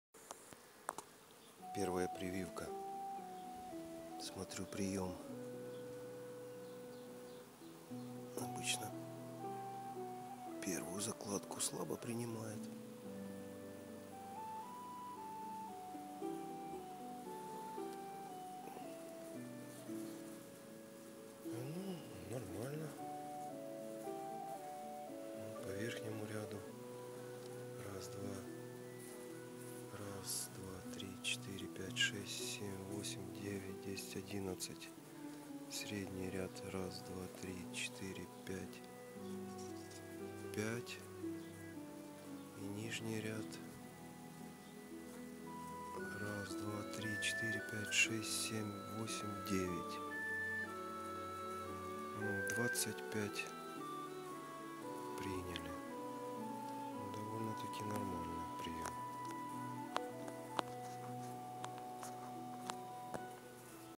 Пчеловодство Вывод маток 2015г Conclusion Beekeeping queens 2015
Вывод маток 2015г.В этом году первую прививку делал 20-го мая ,в это время начала цвести акация.Первый прием личинок хороший ,приняли 25шт.